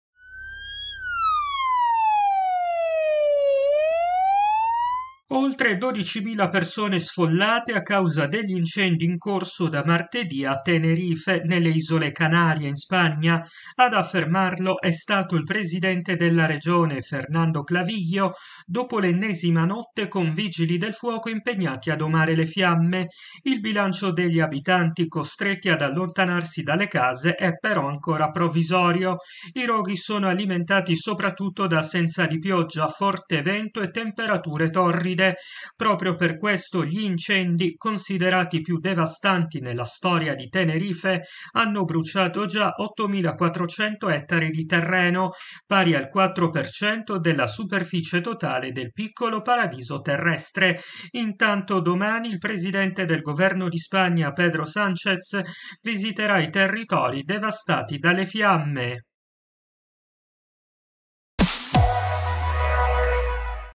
Incendi a Tenerife, oltre 12mila persone sfollate
In pochi giorni, gli incendi hanno bruciato 8.400 ettari di terreno, pari al 4% della superficie totale dell'Isola di Tenerife.

Servizio di Fausto Piu